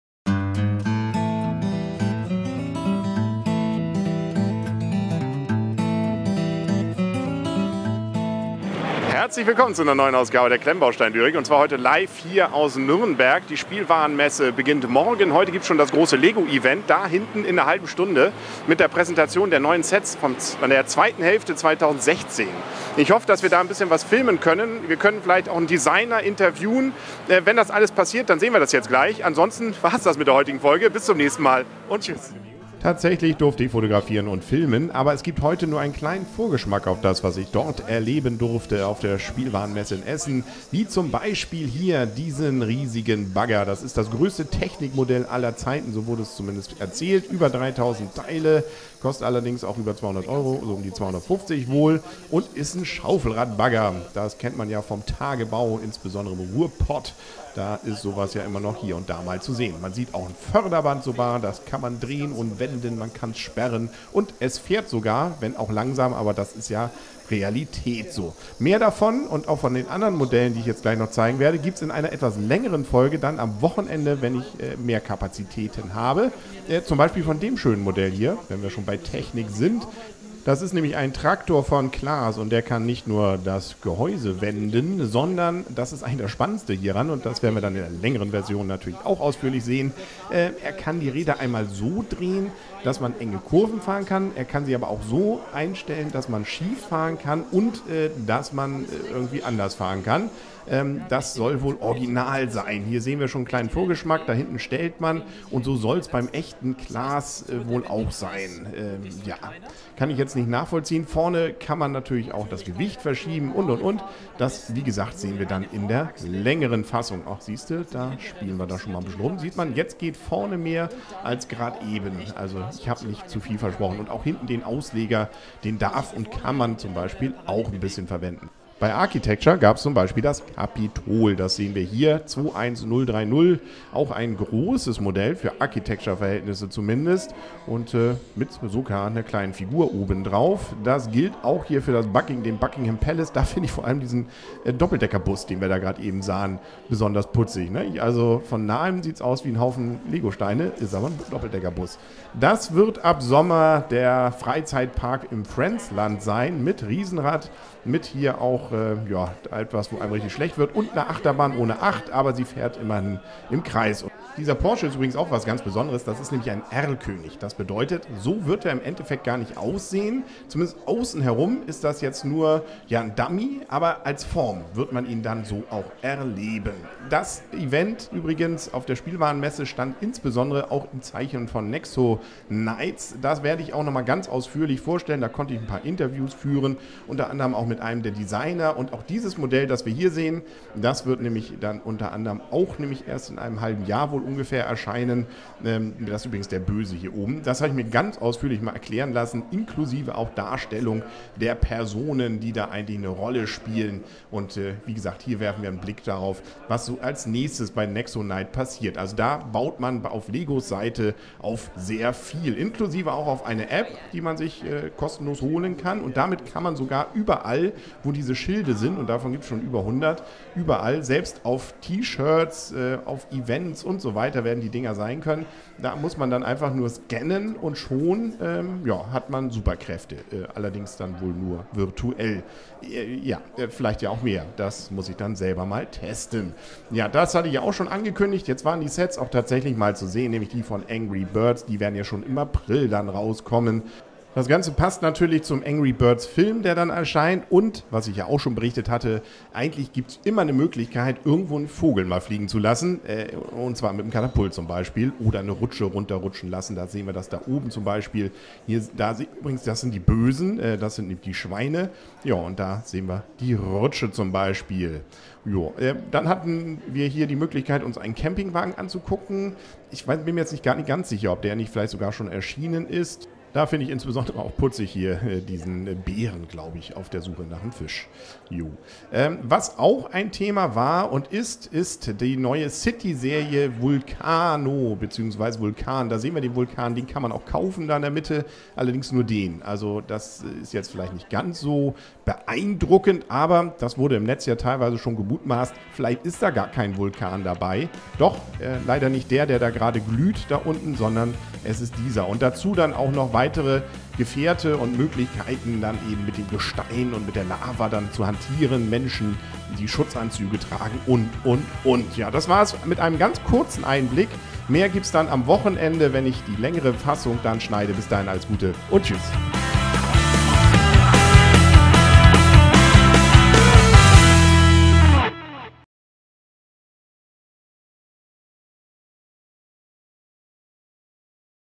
LEGO Neuheiten Sommer 2016 auf der Toy Fair Nürnberg (u.a. 42055 Bucket Wheel Excavator)
Klemmbausteinlyrik vor Ort: LEGO hat viele neue Sets für die zweite Hälfte 2016 präsentiert und die Klemmbausteinlyrik war in Nürnberg auf der Spielwarenmesse und hat sie sich angesehen. Aufgrund der eingeschränkten Kapazitäten im Hotel gibt es heute nur einen kurzen Eindruck u. a. vom 42055 Bucket Wheel Excavator - dem größten Technic-Modell aller Zeiten... Mehr am Wochenende.